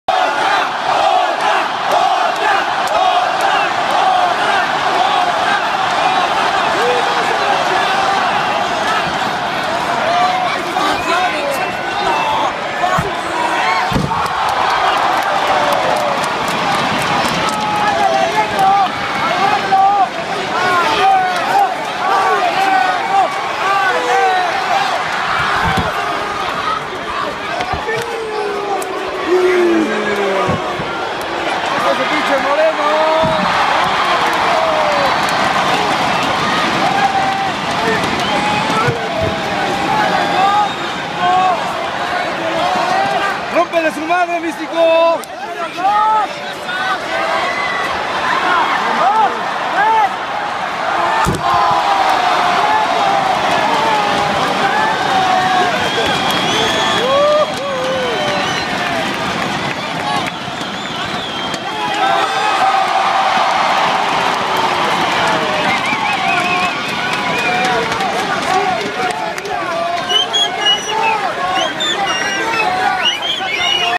Lucha Libre World Cup - Dream Team (MEX) vs ROH (USA)
A part of the match between Rey Mysterio, Alberto el Patron and Myzteziz vs. Brian Cage, Moose and ACH.

Una parte de la pelea entre Rey Mysterio, Alberto el Patron y Myzteziz vs. Brian Cage, Moose y ACH.